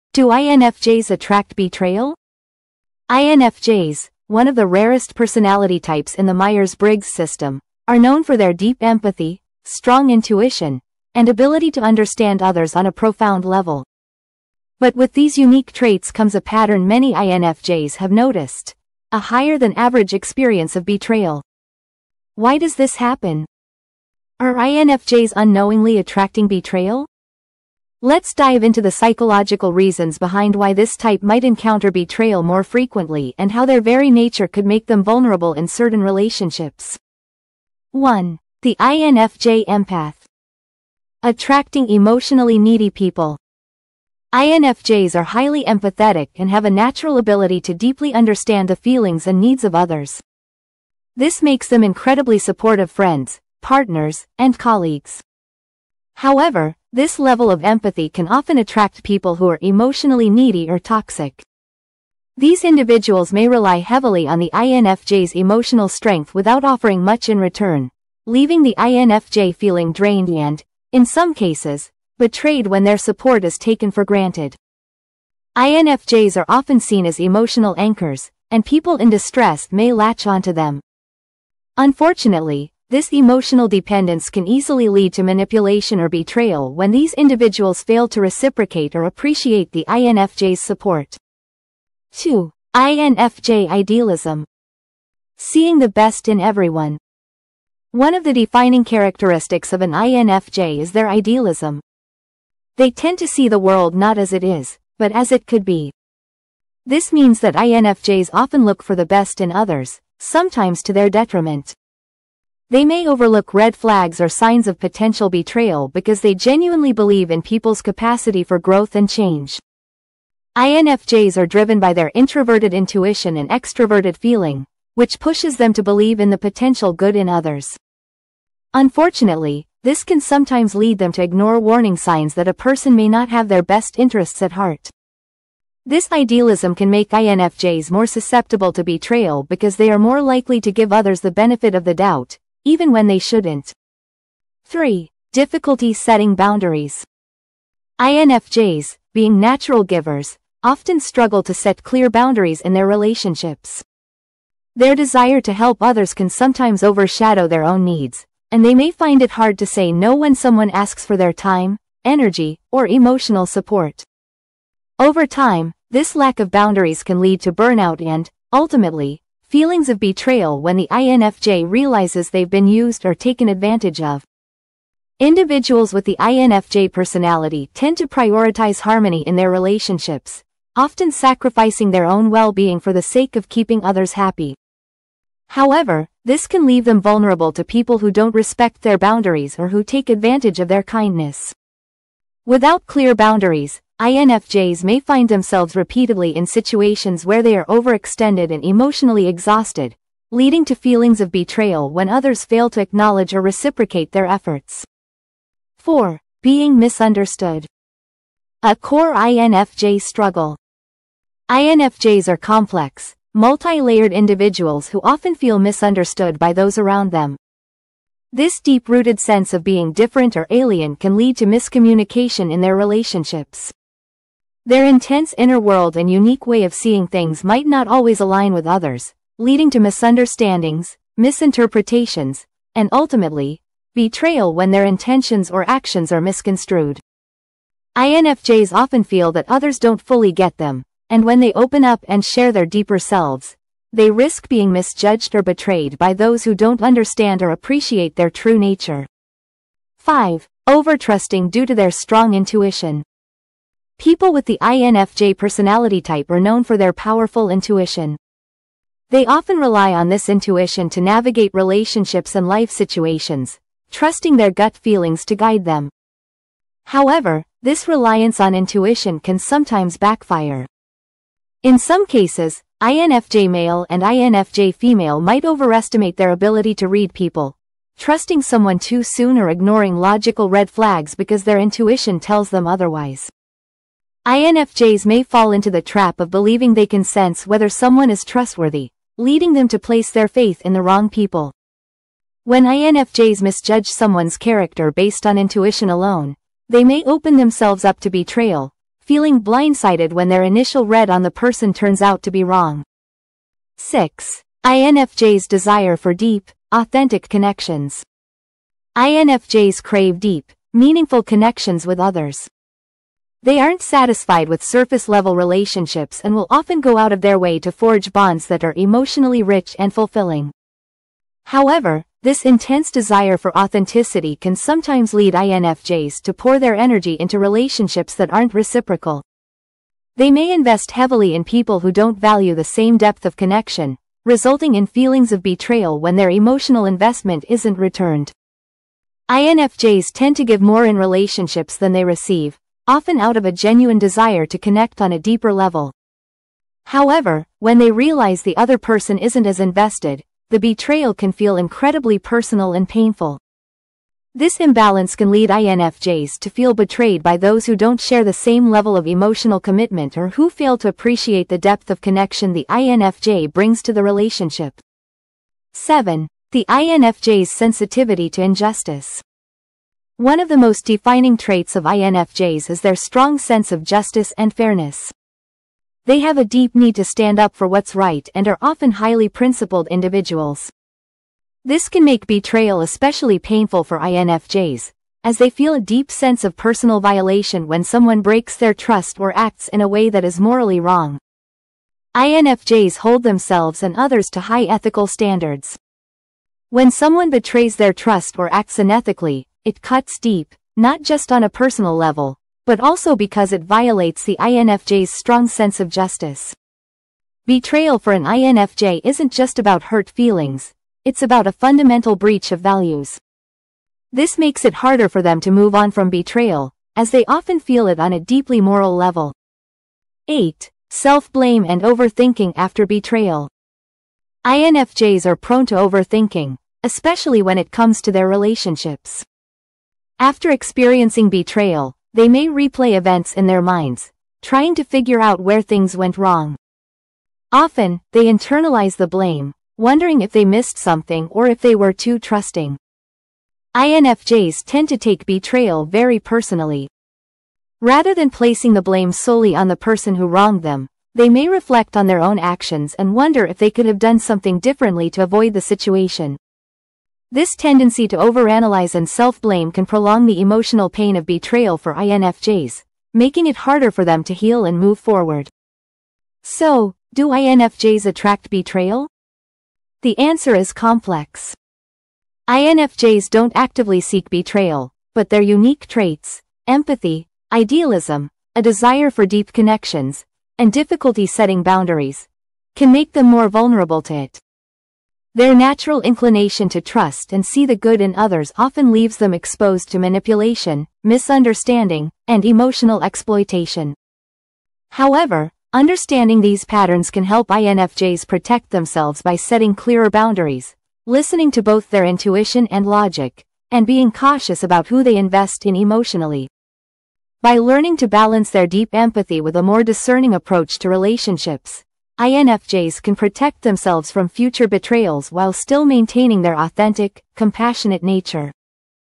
Do INFJs Attract Betrayal?
The INFJ personality type is one of the rarest personality types in the Myers-Briggs personality type. INFJ male and INFJ female are known for their deep empathy, strong intuition, and ability to understand others on a profound level. But with these unique traits comes a pattern many INFJs have noticed: a higher-than-average experience of betrayal. Why does this happen? Are INFJs unknowingly attracting betrayal? Let’s dive into the psychological reasons behind why this MBTI type might encounter betrayal more frequently and how their very nature could make them vulnerable in certain relationships.